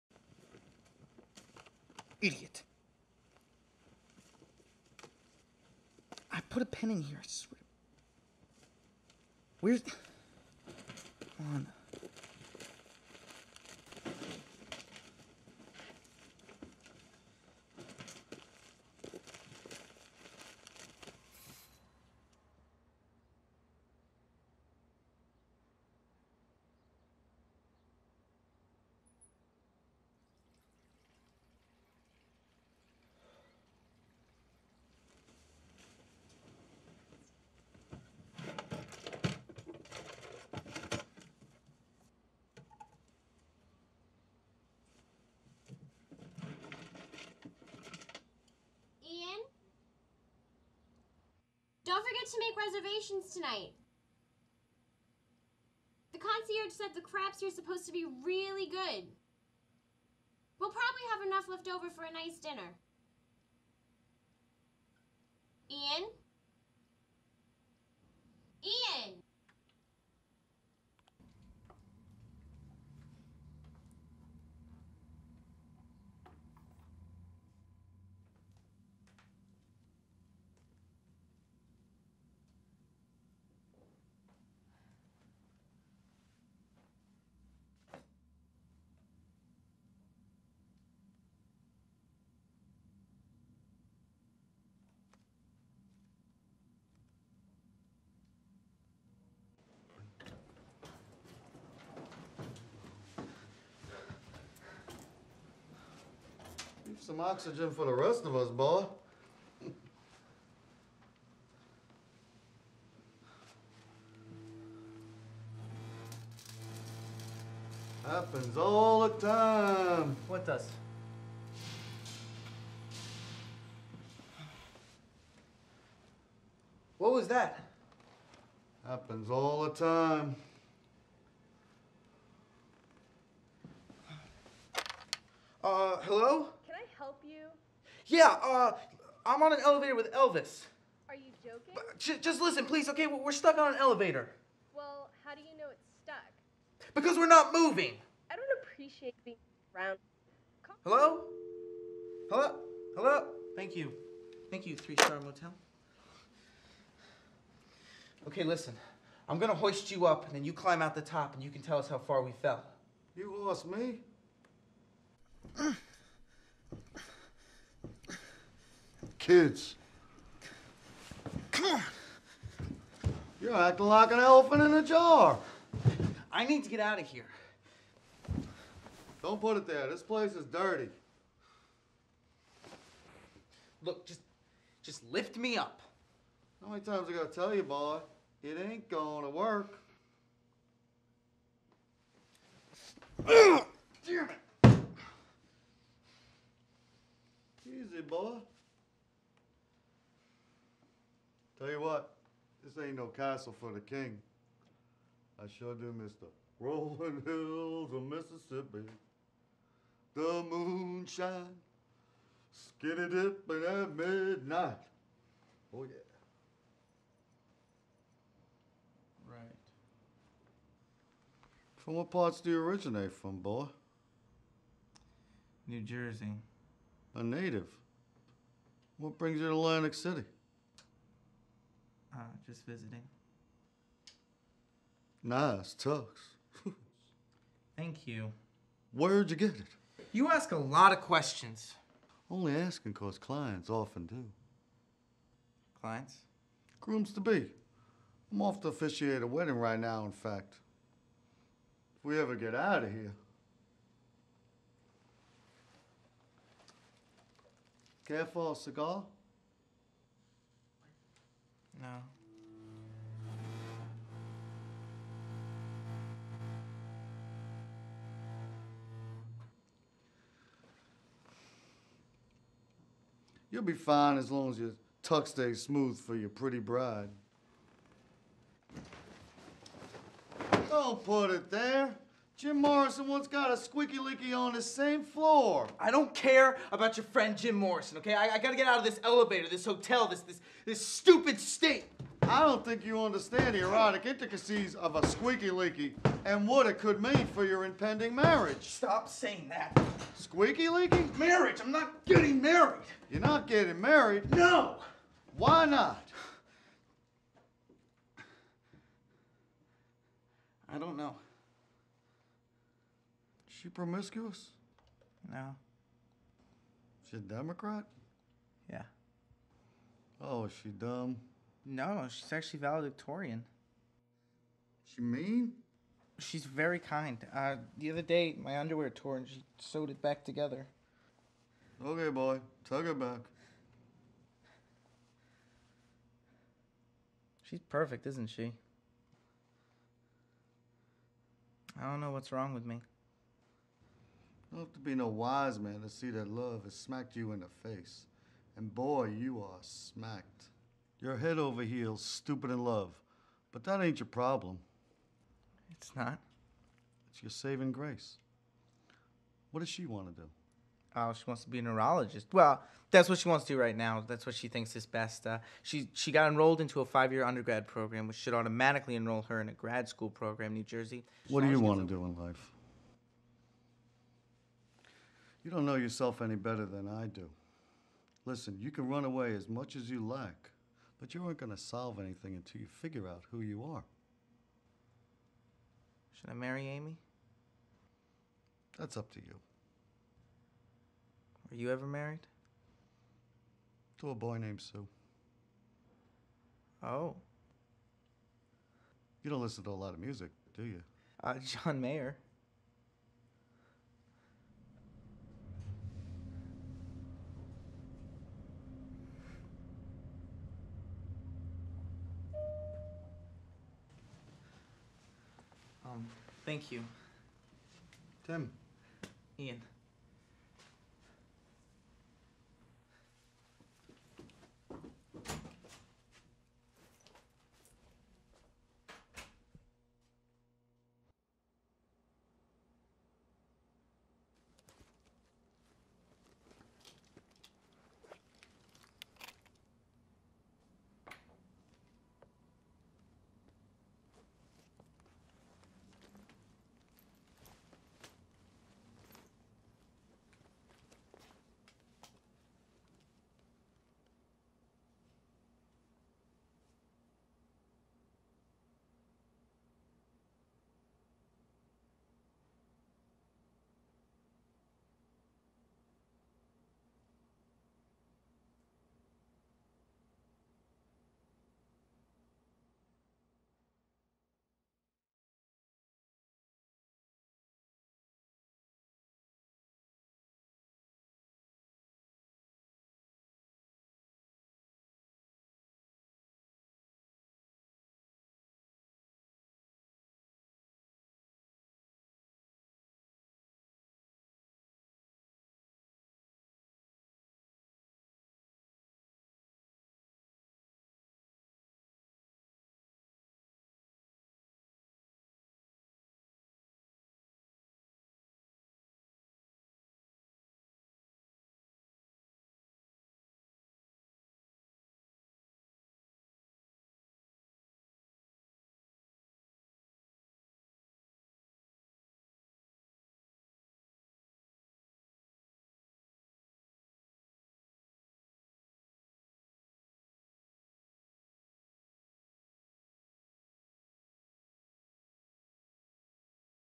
Church Bell Rock
A panicked groom has a chance meeting with an Elvis impersonator when their hotel elevator becomes stuck.

I filmed this for Tzipi Trope's Spring 2009 Color-Sync Workshop. It was my last project before graduating.